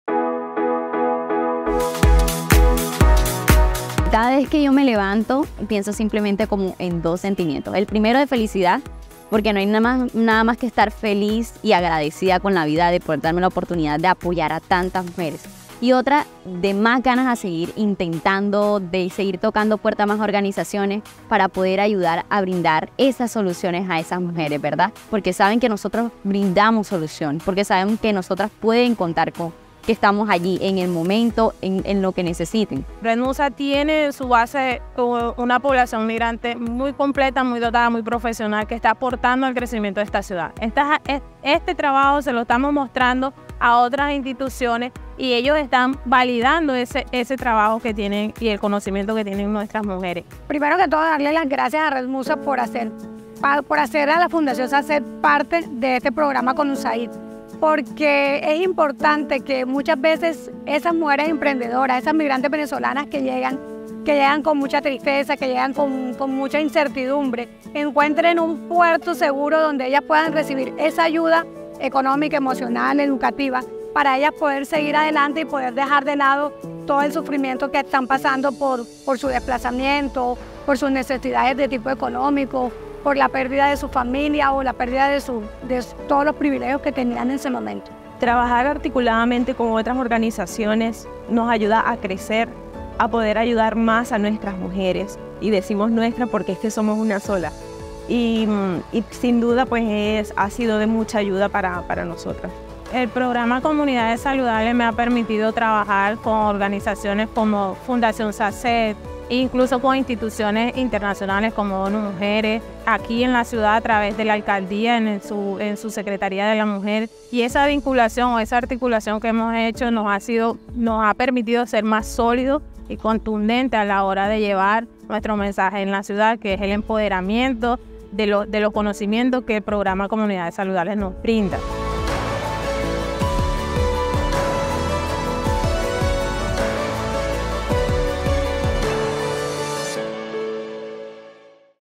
Fundación REDMUSA impacta la vida de las mujeres migrantes venezolanas y colombianas en Santa Marta
Descubre cómo la Fundación REDMUSA, a través de la articulación estratégica con el Programa Comunidades Saludables de USAID, transforma e impacta la vida de la población migrante venezolana, colombianos retornados y comunidades de acogida en Santa Marta, Colombia.

English Translation
Uncover the remarkable impact of the REDMUSA Foundation, bolstered by strategic collaborations with LHSS Colombia or USAID's Comunidades Saludables Programa. Witness how they're transforming the lives of Venezuelan migrants, Colombian returnees, and host communities in Santa Marta, Colombia.